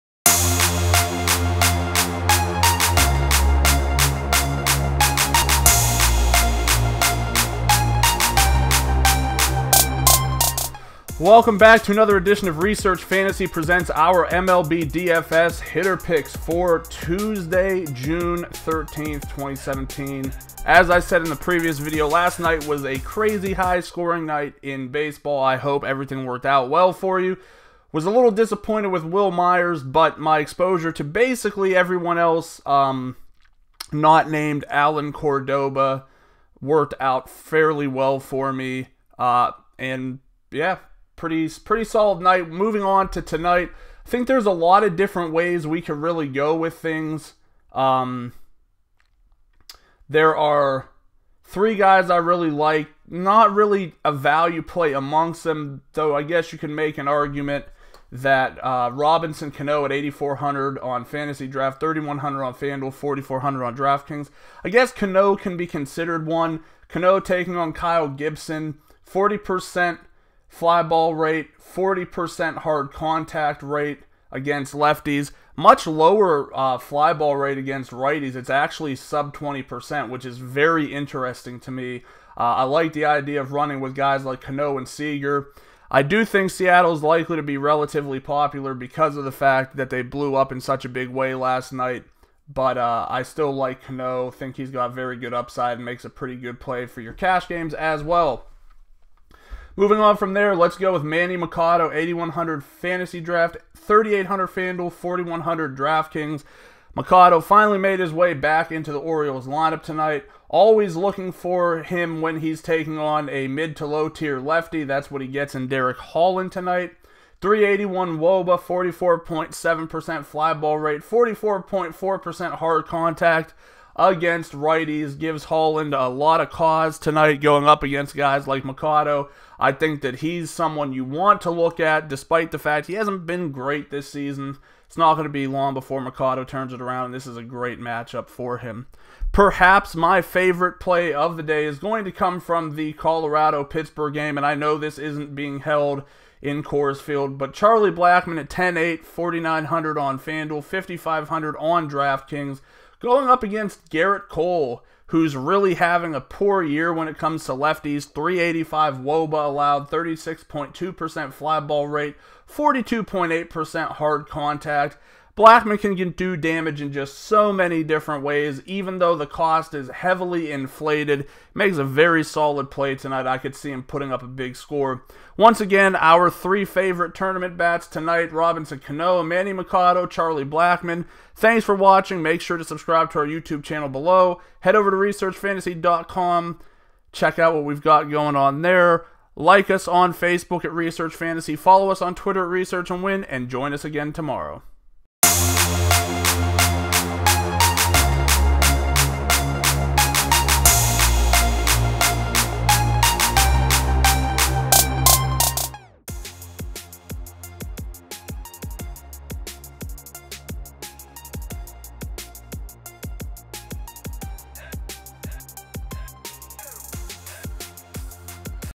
MLB FanDuel and DraftKings DFS Hitting Picks - 6/13/17
Nick breaks down his top plays for tonight's slate on FanDuel and DraftKings, including Robinson Cano, Manny Machado and Charlie Blackmon.